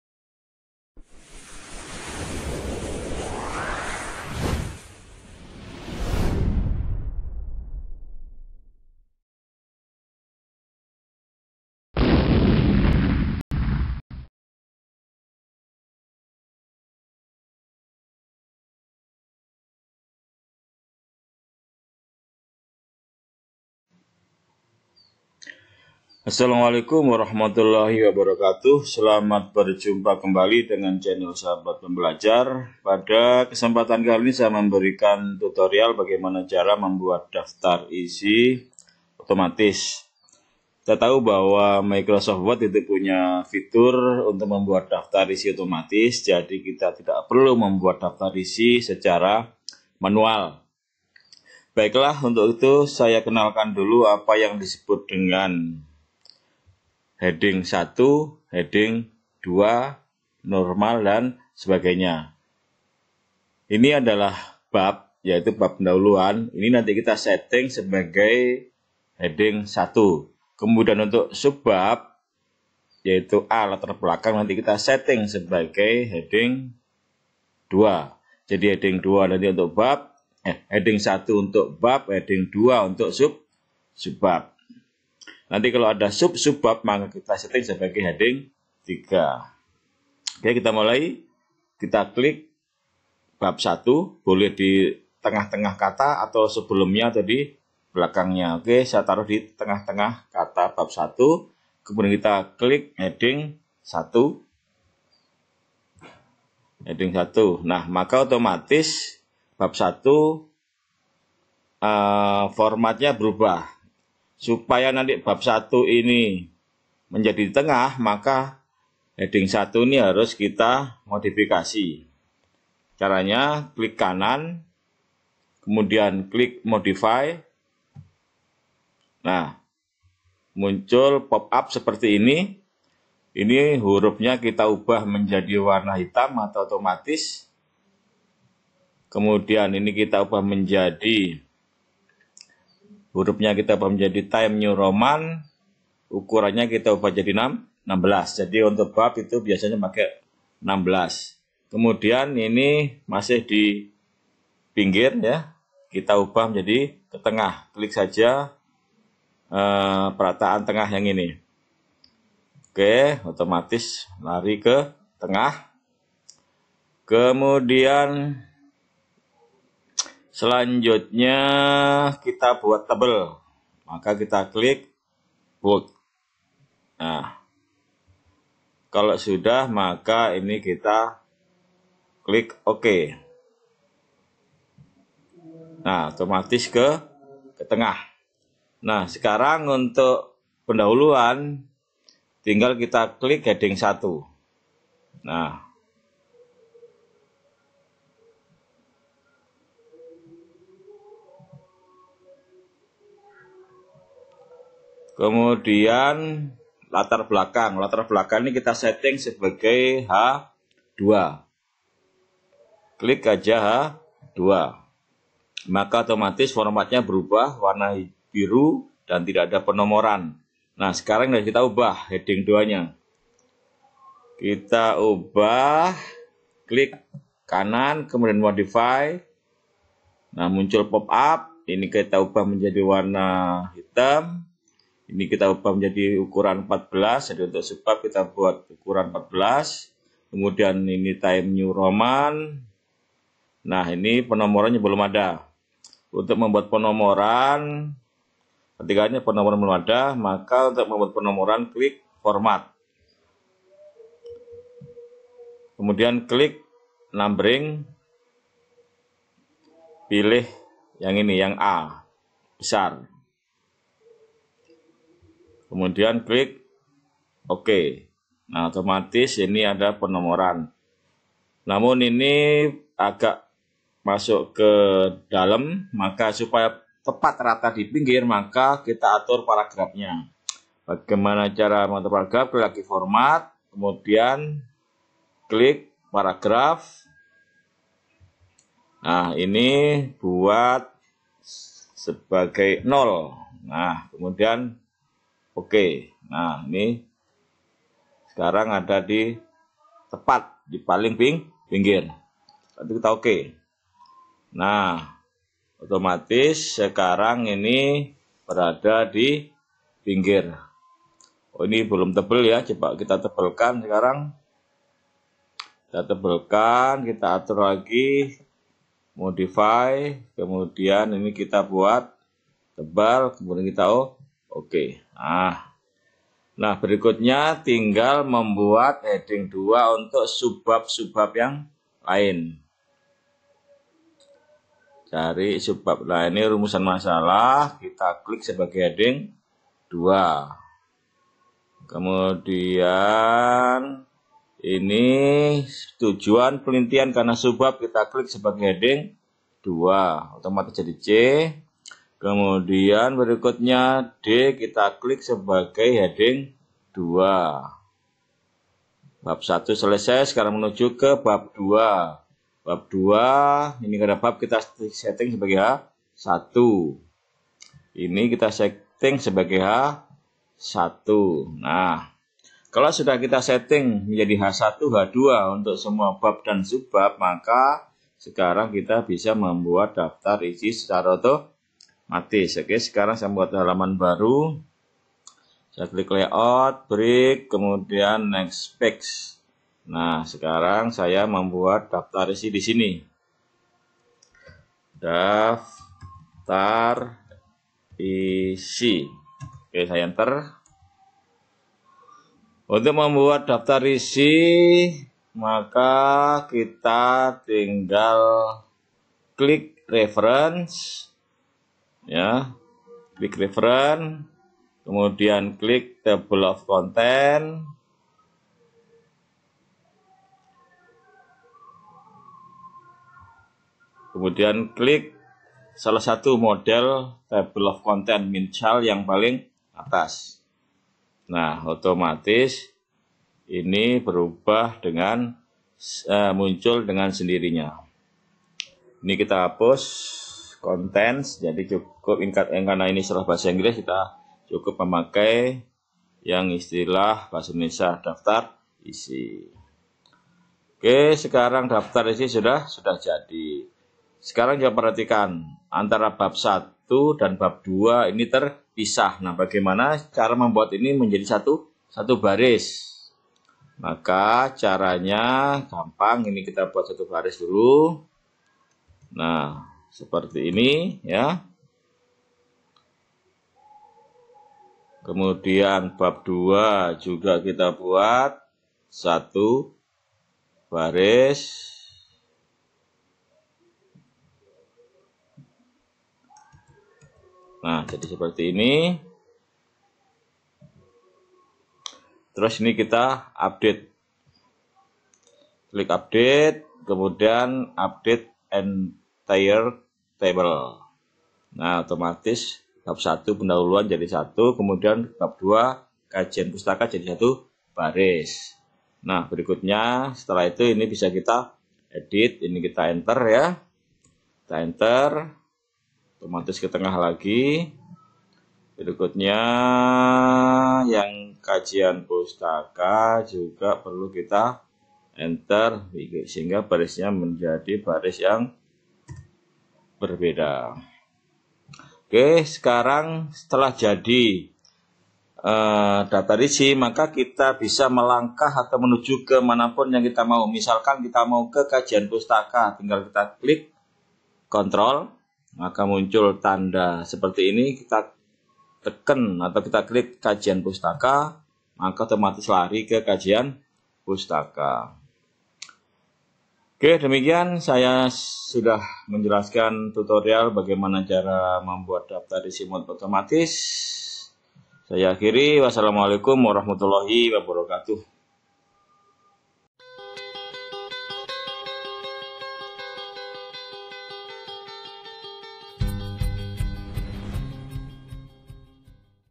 CARA MUDAH MEMBUAT DAFTAR ISI OTOMATIS
Membuat karya tulis, skripsi, tesis, disertasi, makalah, artikel, laporan penelitian, buku dan lain-lain, daftar isi adalah salah satu komponen yang harus ada. Daftar isi dapat dibuat secara otomatis. Di vidoe ini dijelaskan cara mudah membuat daftar isi secara otomatis. Daftar isi otomatis dapat dibuat untuk tulisan apapun baik makalah, artikel, buku, laporan penelitian, prosiding, skripsi, tesis, disertasi dan lain-lain.

video ini menjelaskan cara mudah membuat daftar isi otomatis menggunakan microsoft word.